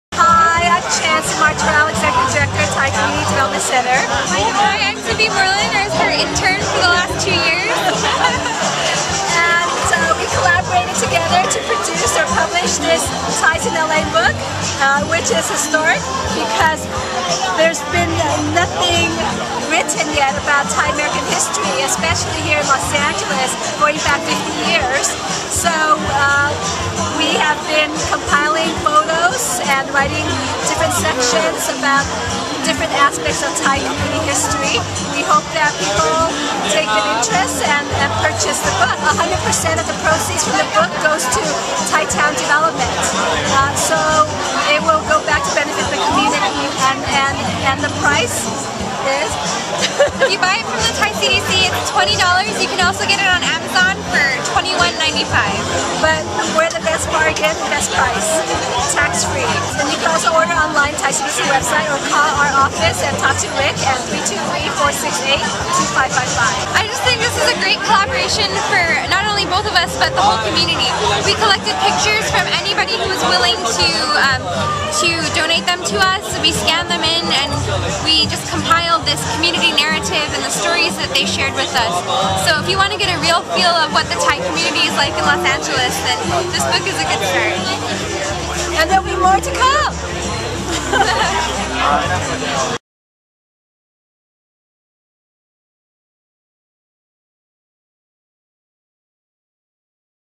'Thais In Los Angeles' book authors at 2011 Songkran Thai New Year Festival
Chancee Martorell and Tippe Morlan, who published the new Arcadia Images of America book, "Thais In Los Angeles," talk about the book at the 2011 Songkran Thai New Year Festival in Thai Town on April 3.